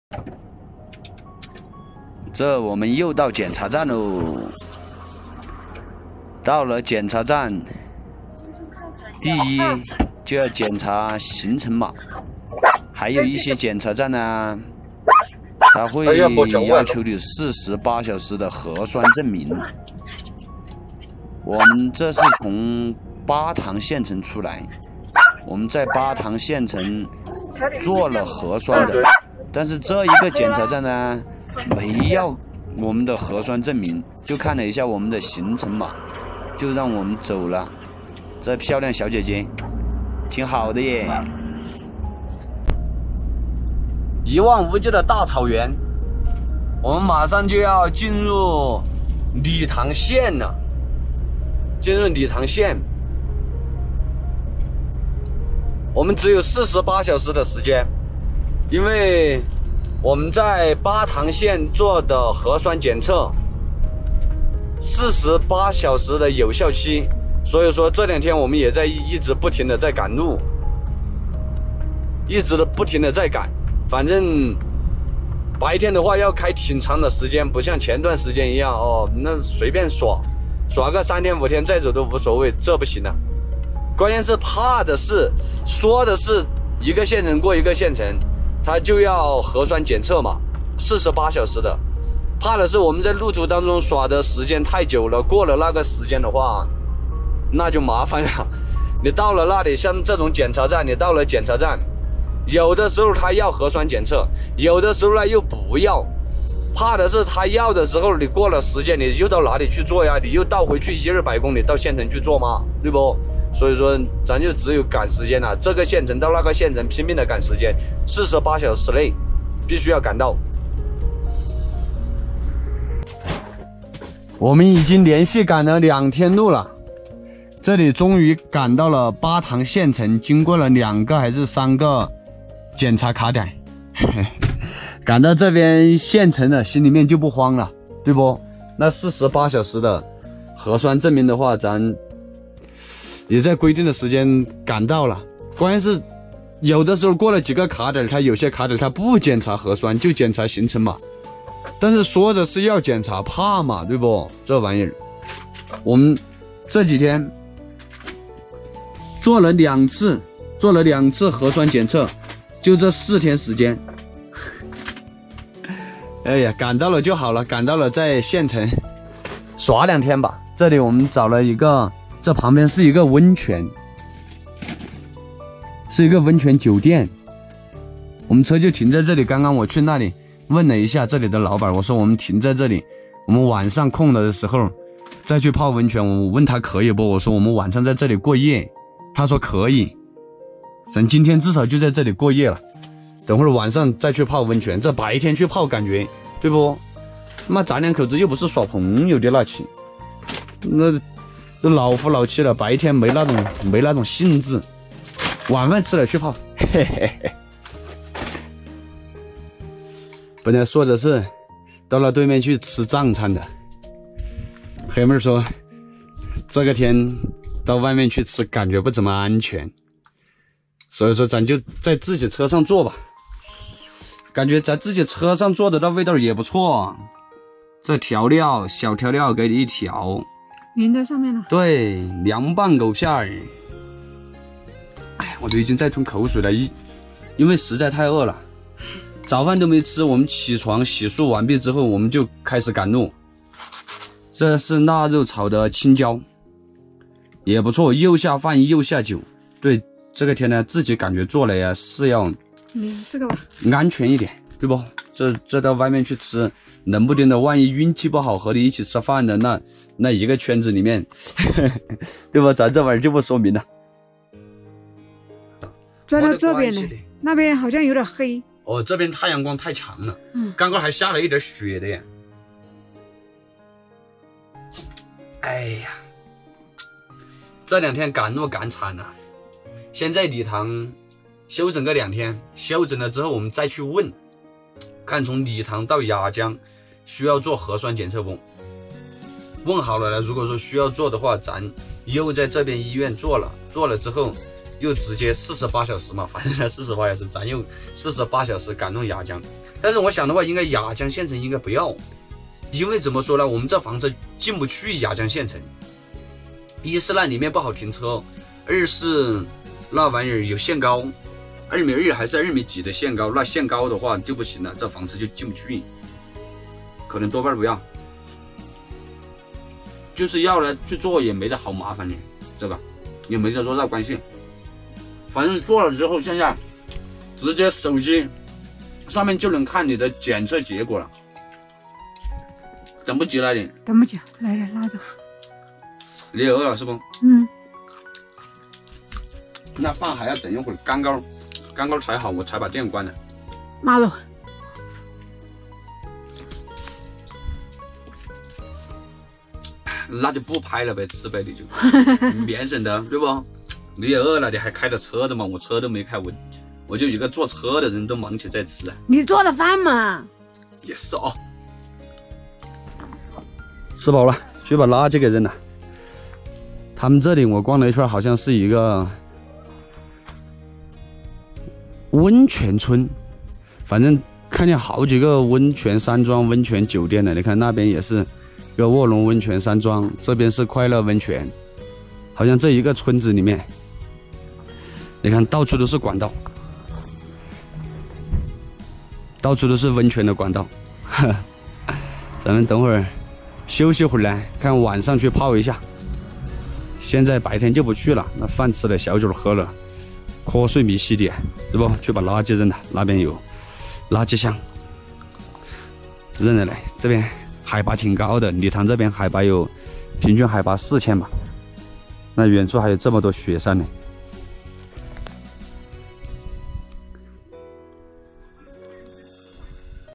接到通知房车48小时内必须开出县城，连续赶两天路，一刻不敢停留【868房车旅行】
80后夫妻记录房车生活
每天更新旅行见闻，房车生活
Update travel information every day, RV life
更多精彩视频